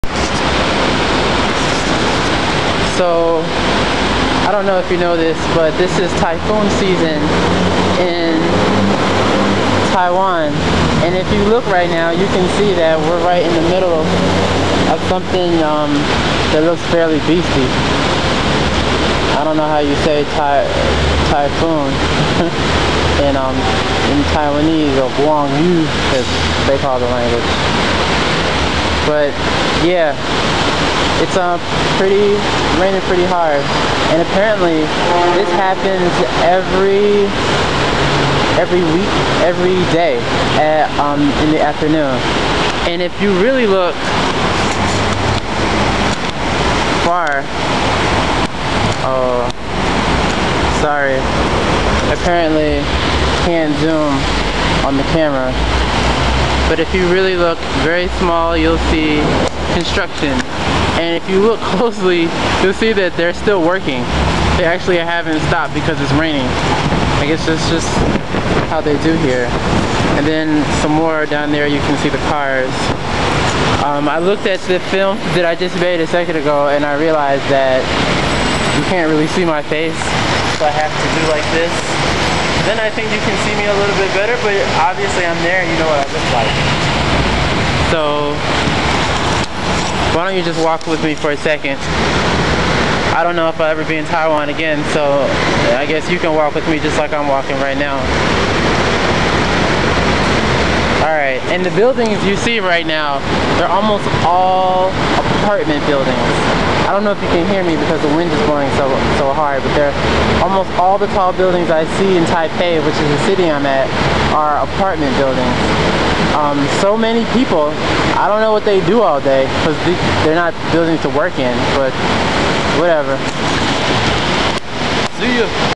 typhoon 1
This is a Taipei storm during typhoon season